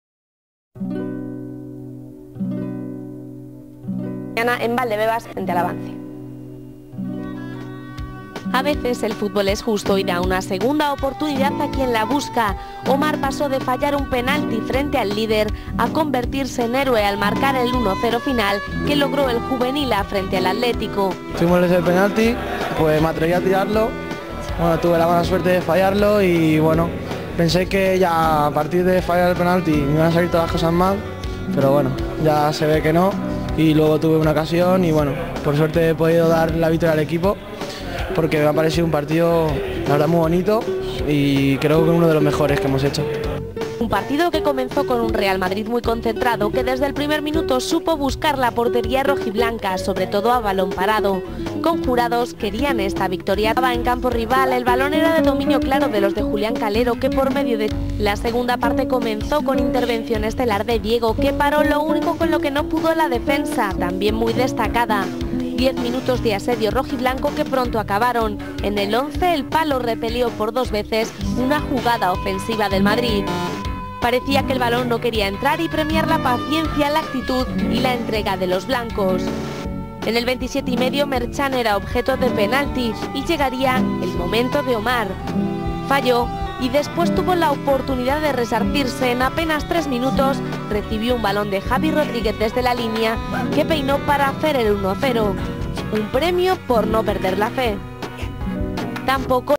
golazo de Omar al Atletico de Madrid
Omar es uruguayo juega en el Juvenil A del real Madrid